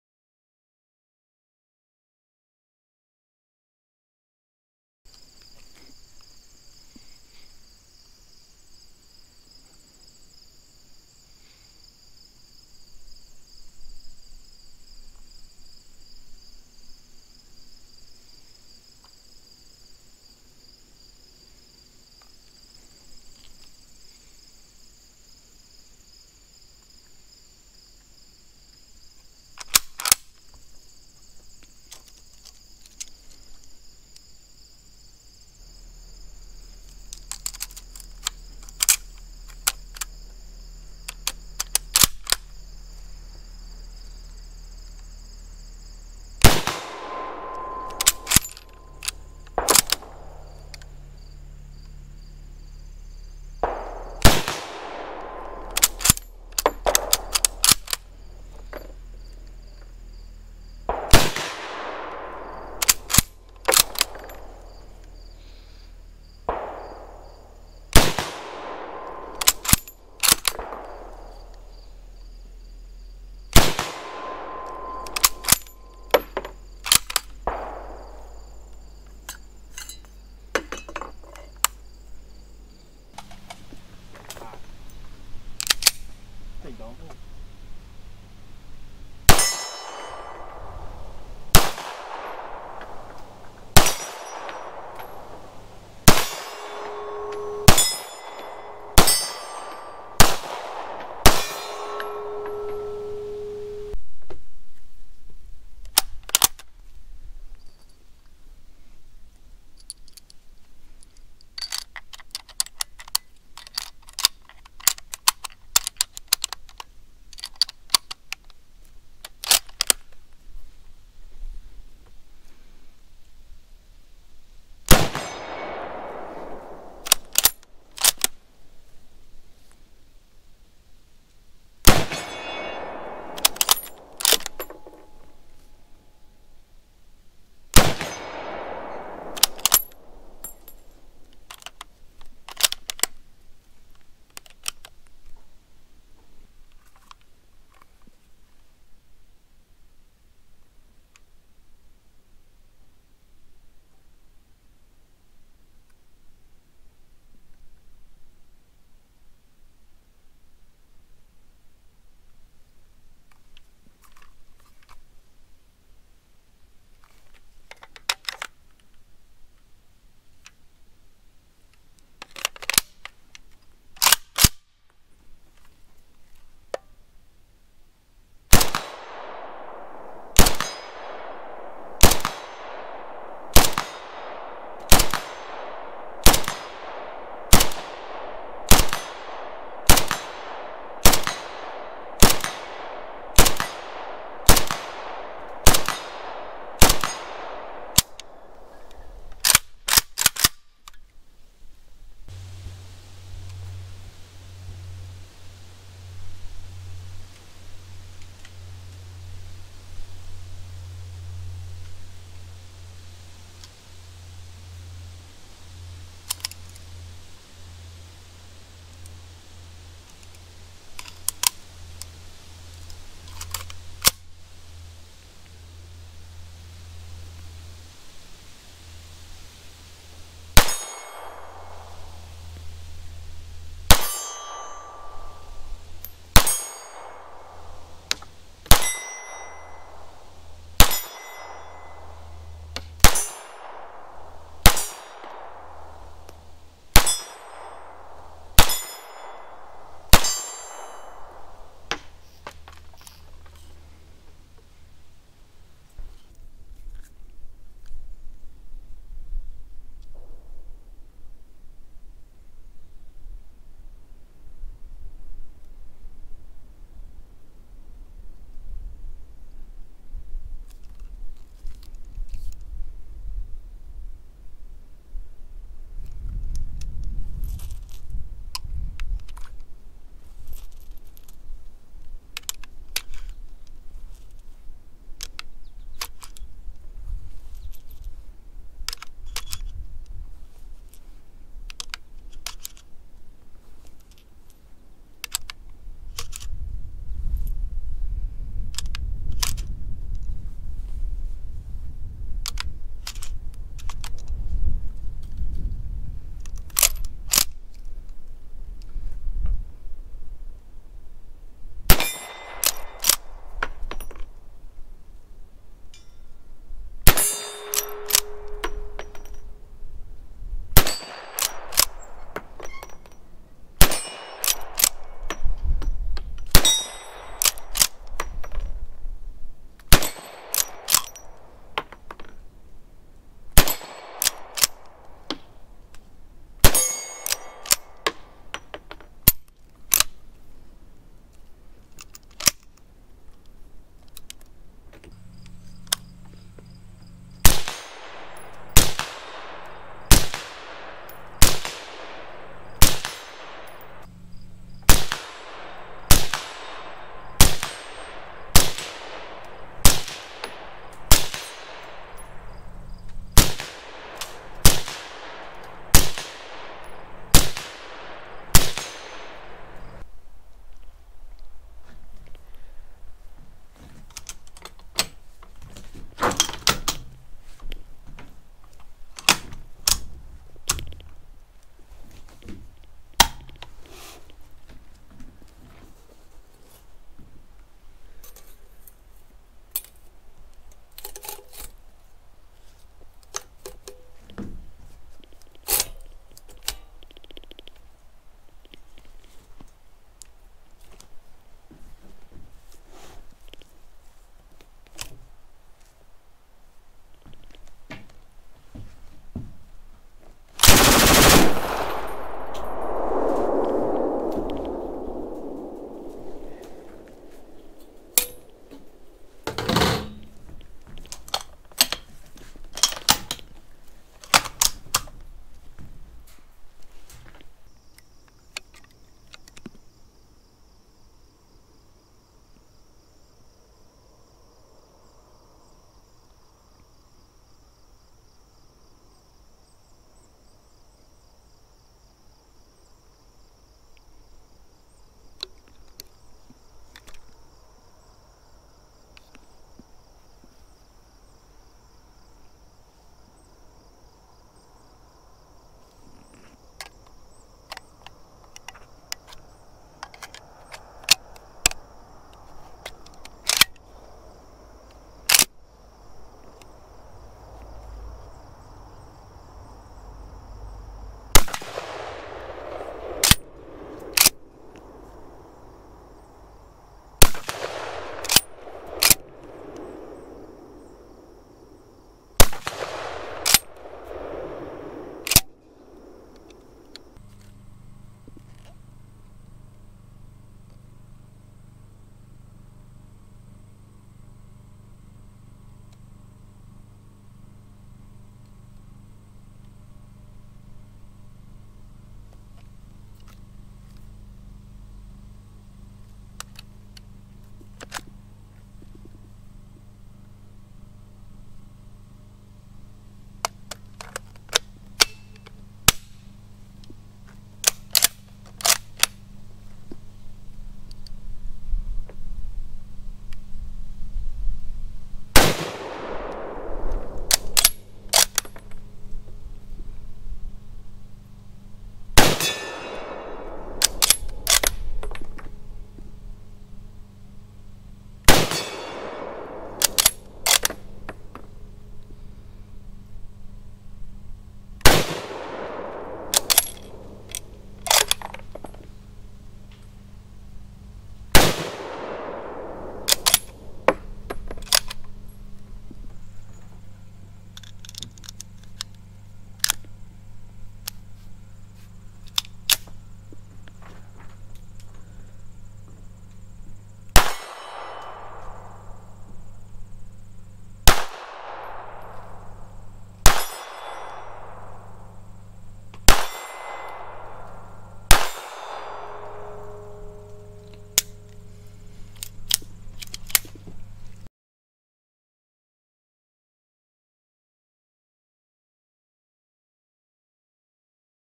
GoPro Shooting Guns Compilation Part 6
Shooting a different variety of weapons all filmed with a GoPro camera. Can you name all of them?